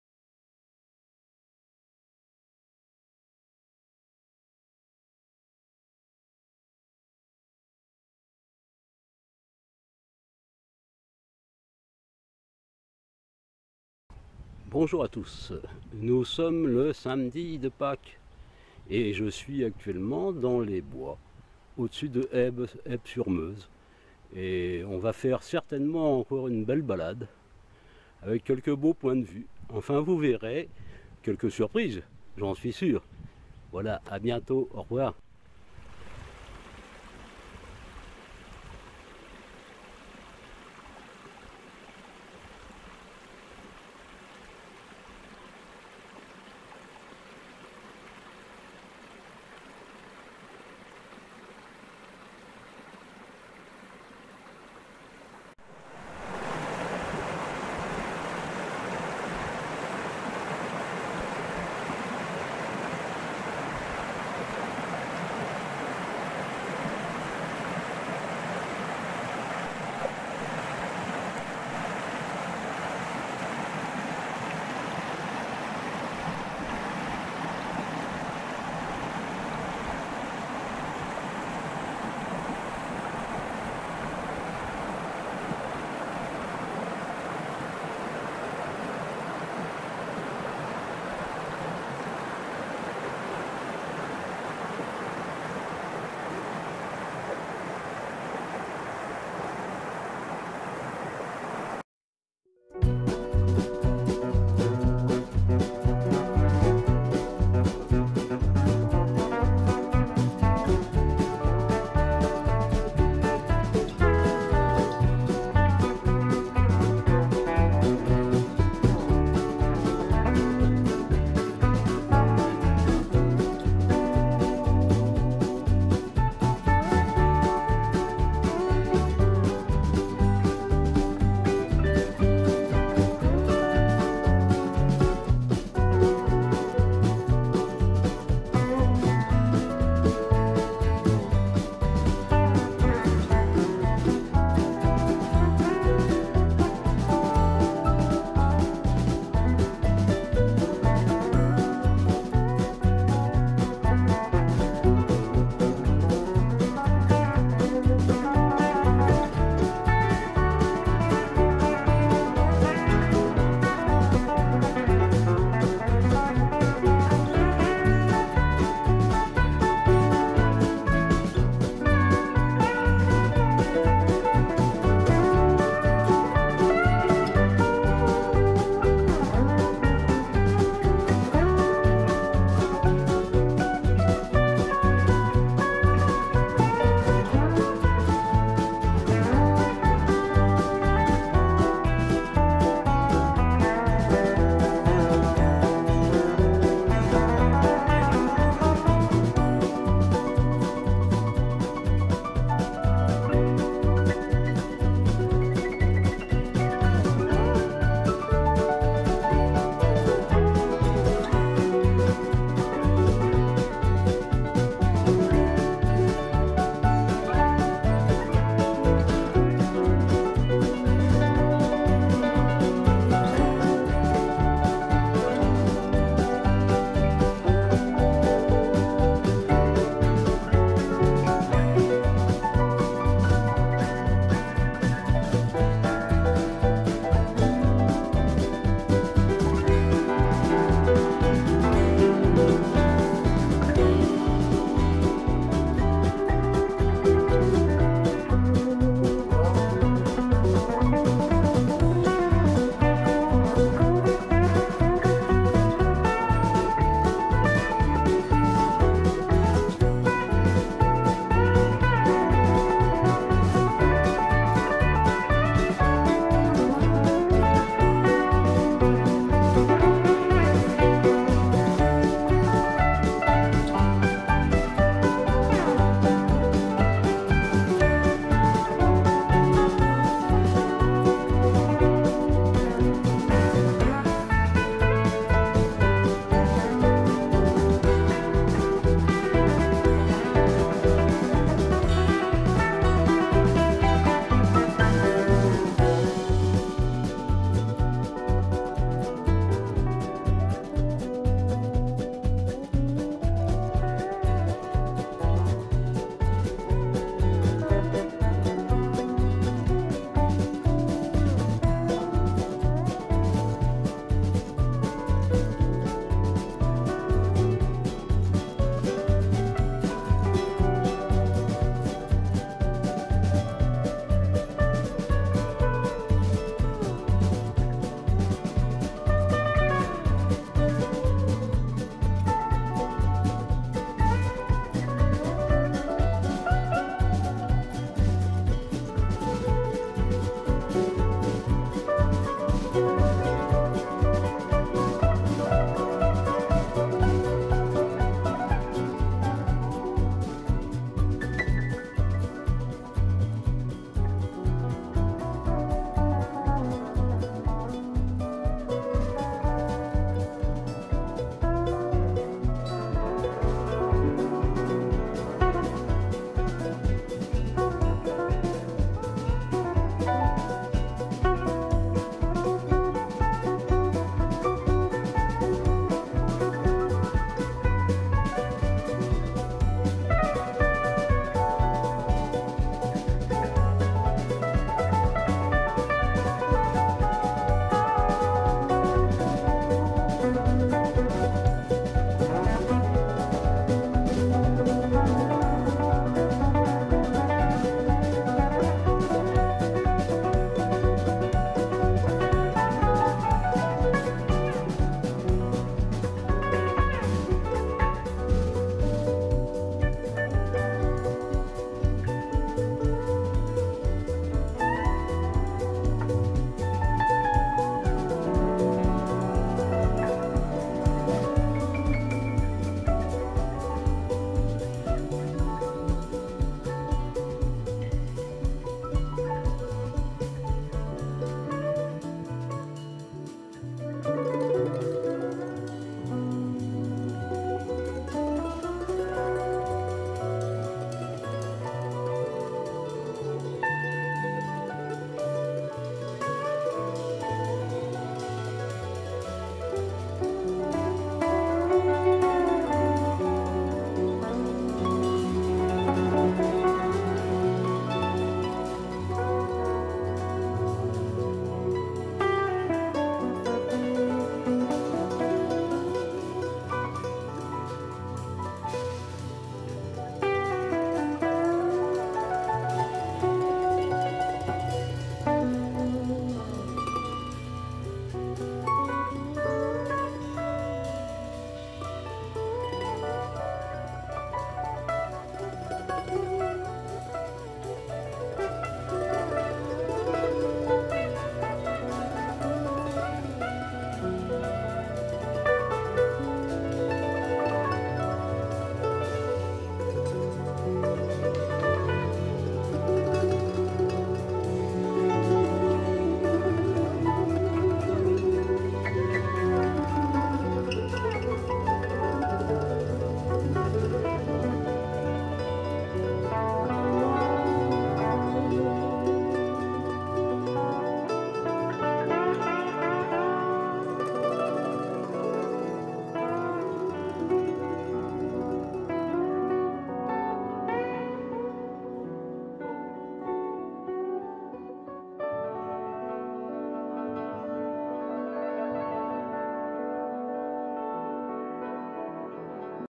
Haybes-la-Jolie
Une bien belle randonnée, à travers bois et sentiers forestiers, en partant de Haybes-sur-Meuse dite aussi Haybes-la-Jolie ou Haybes-la-Rose, via l'ardoisière, les grottes de Madame de Cormont, les hesses ou hêtres, pour terminer ce parcours difficile sur le site des parapentes qui domine la magnifique ville de Fumay et ses boucles de Meuse.